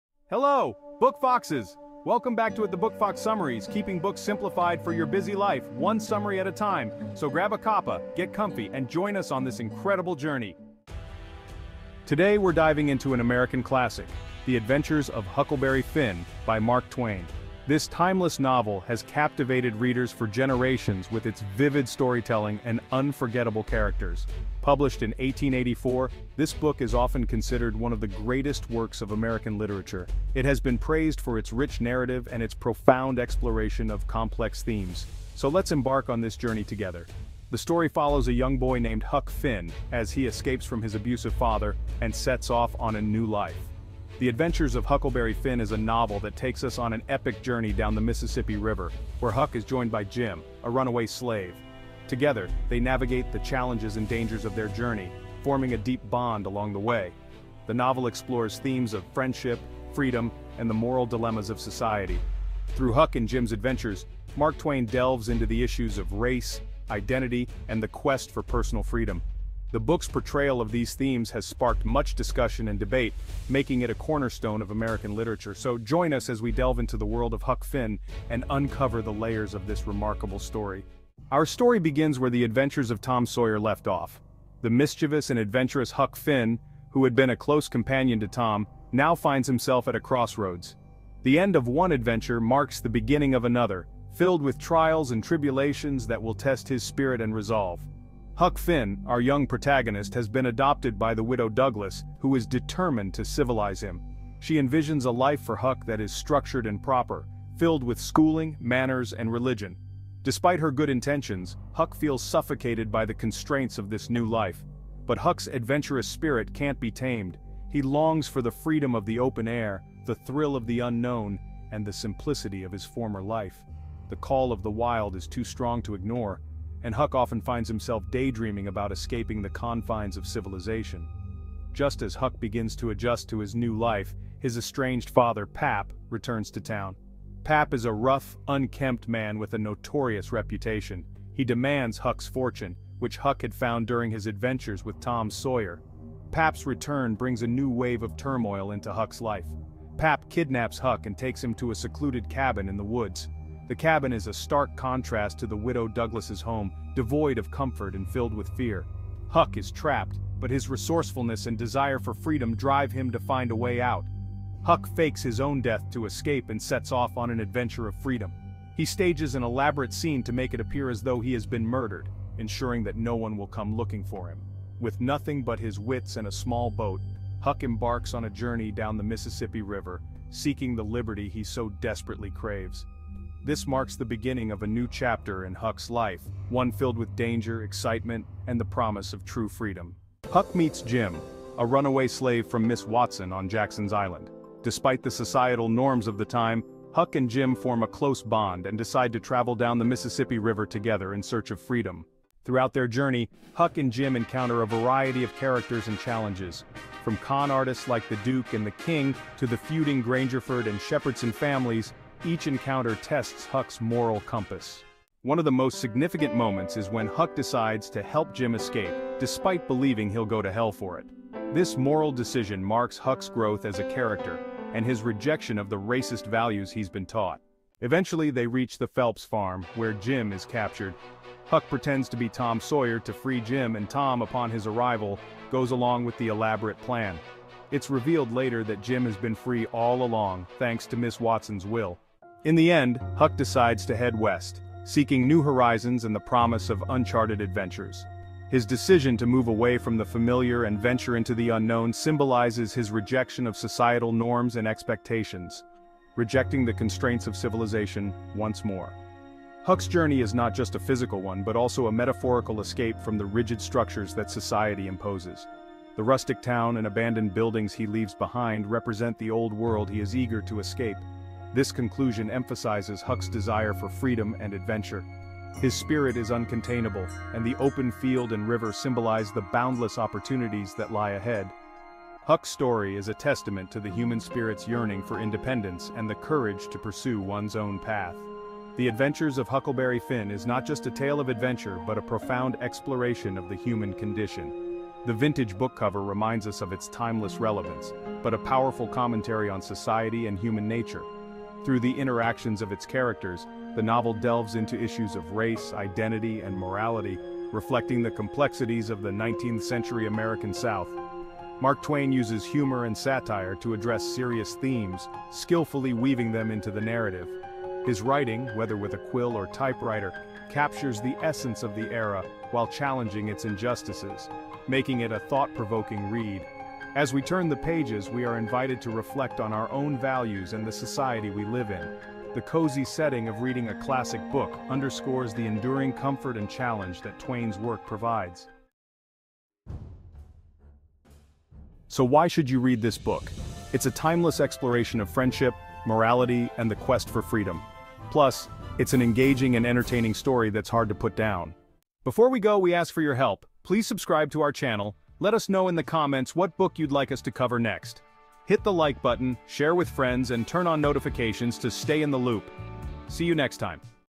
Why Huckleberry Finn is More Relevant Than Ever
Hello, Book Foxes! 📚 Welcome back to @thebookfoxsummaries, where we keep Books Simplified for Your Busy Life, One Summary at a Time!

Today, we're taking you on an exciting journey down the Mississippi River with Huck Finn in Mark Twain's timeless classic, "The Adventures of Huckleberry Finn." This novel, first published in 1884, remains one of the greatest works of American literature, celebrated for its rich storytelling and profound themes.

📖 What You'll Learn:

Plot Overview: We'll start with a brief recap of the story, beginning with Huck's escape from his abusive father and his adventurous journey with Jim, a runaway slave. Follow their exploits, encounters with various characters, and the challenges they face as they seek freedom.
Key Themes: Discover the critical themes of the book, such as friendship, freedom, and the moral dilemmas Huck encounters. We'll explore how Twain uses satire and humor to critique society and highlight the complexities of human nature.
Character Analysis: Get to know the main characters, including Huck Finn, Jim, Tom Sawyer, and the infamous conmen, the Duke and the King. Learn how their interactions shape the narrative and Huck's development.
Memorable Quotes: We'll share some of the most impactful quotes from the book, including the famous line, "All right, then, I'll go to hell," which signifies Huck's moral growth and his commitment to doing what he believes is right.
Moral Growth: Understand Huck's internal struggles and how he evolves throughout the story, challenging the racist attitudes of his time and forging his own path.
Relevance Today: We'll discuss why "The Adventures of Huckleberry Finn" continues to be relevant today, offering insights into societal issues and human behavior that resonate with modern readers.
✨ Why Watch This Video:
Whether you're a student, a literature enthusiast, or just someone looking for a good story, this summary provides a comprehensive look at "The Adventures of Huckleberry Finn." It's perfect for those who want to understand the book's essence without reading the entire novel.

OUTLINE:
00:00:00 Introduction
00:00:10 Overview of 'The Adventures of Huckleberry Finn'
00:01:21 Huck's Early Struggles
00:03:10 Meeting Jim
00:03:22 Journey Down the Mississippi
00:03:35 Huck's Moral Dilemma
00:03:47 The Phelps Farm
00:04:02 Conclusion and Themes
00:05:38 Why Read 'The Adventures of Huckleberry Finn'?
00:05:51 Closing Remarks